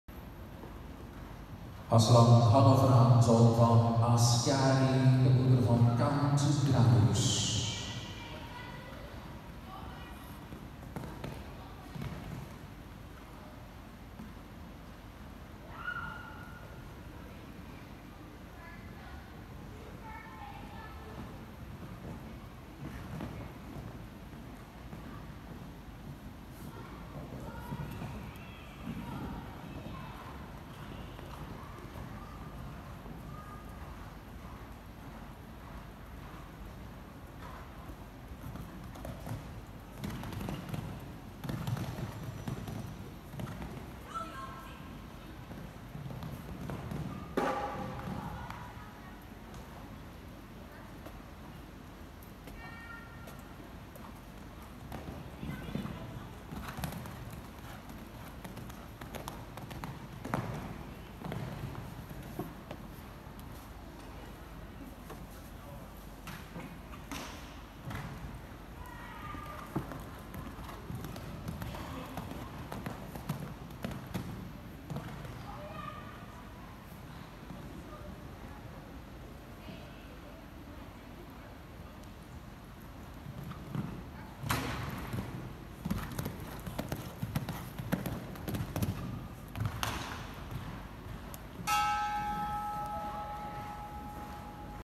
Aslan fsa cup sentower 6 januari 2018
Aslan gelding by Askari x Count Grannus Born 2011 rider robbert Ehrens